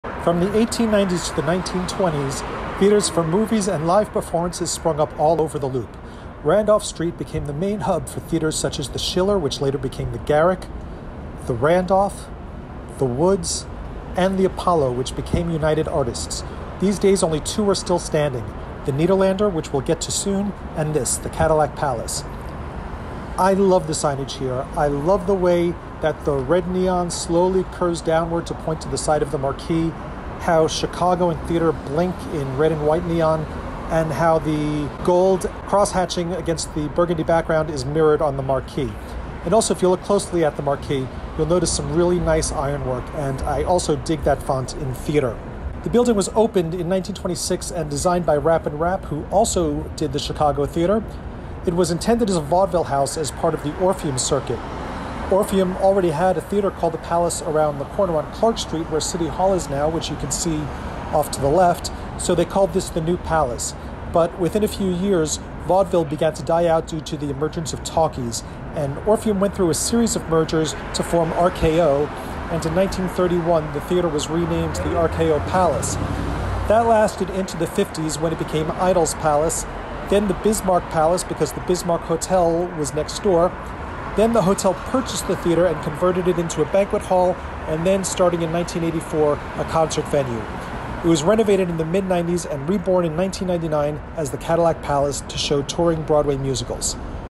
Cadillac Palace Theatre
Learn about the history of Chicago's Cadillac Palace Theatre and its sign.